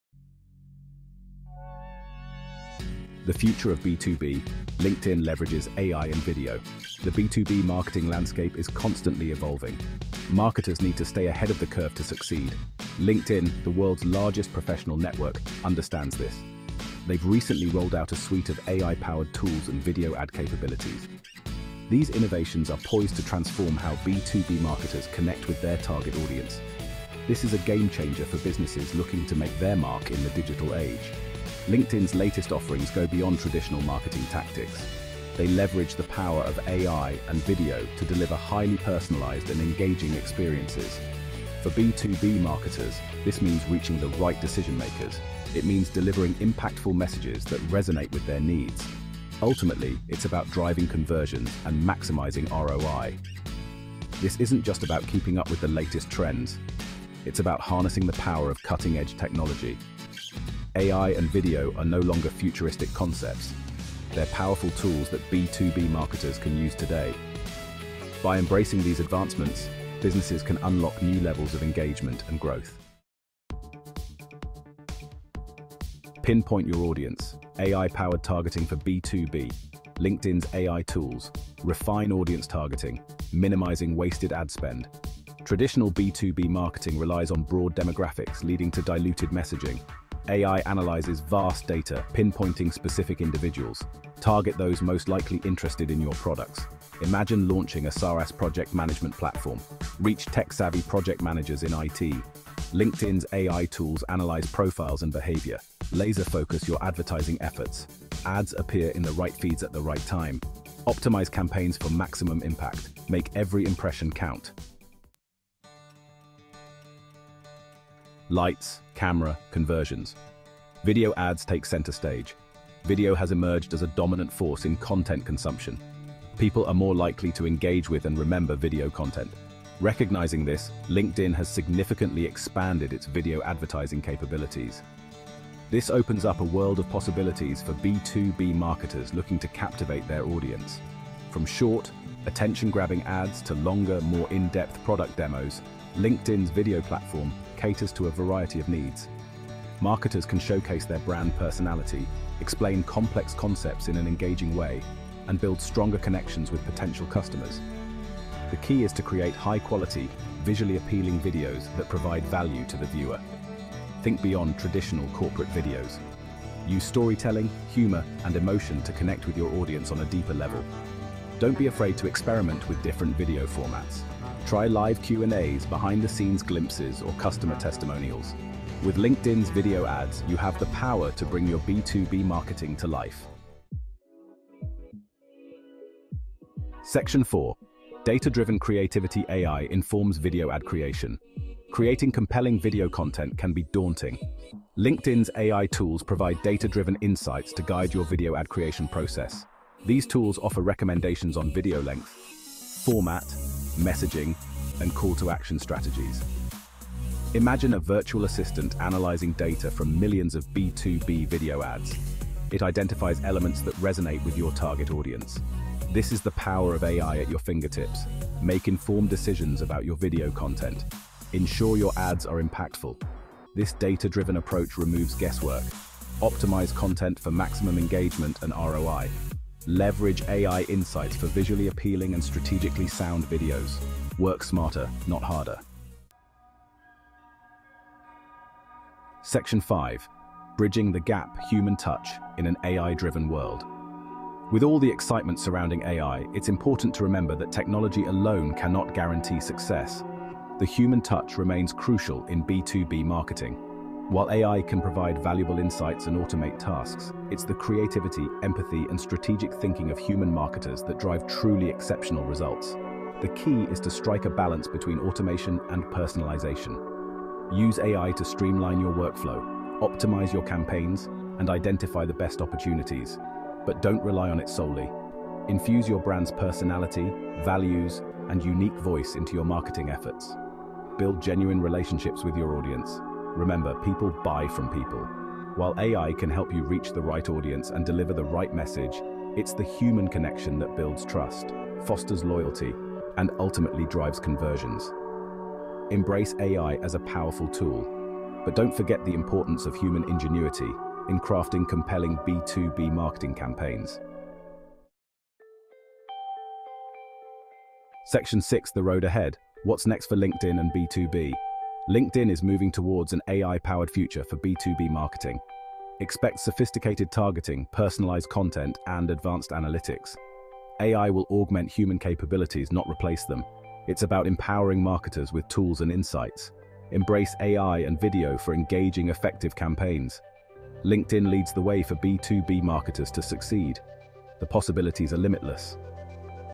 AI-Driven Video Ads for B2B Marketers
In this deep dive, we explore the significant impact these innovations have on targeting precision and campaign optimization, allowing marketers to connect with their audience more effectively than ever before.

We also provide actionable tips for creating engaging, high-quality videos on LinkedIn that resonate with your target demographic. Plus, understand the importance of maintaining a human touch in an increasingly automated world to build trust and drive conversions.

Don't miss out on leveraging these groundbreaking advancements for your business.

OUTLINE:

00:00:00 LinkedIn Leverages AI and Video
00:01:12 AI-Powered Targeting for B2B
00:01:55 Video Ads Take Center Stage
00:03:03 AI Informs Video Ad Creation
00:03:56 Human Touch in an AI-Driven World
00:05:04 What's Next for LinkedIn and B2B?